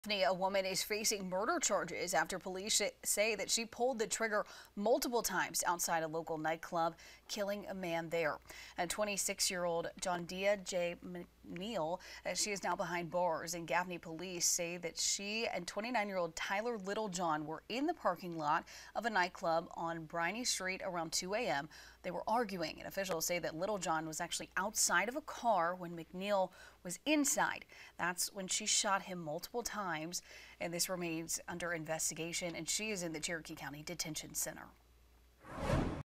Suspect charged with murder after Gaffney man shot in parking lot
Officers charged 26-year-old Jondaijah Driana McNeil with murder and possession of a weapon during the commission of a violent crime.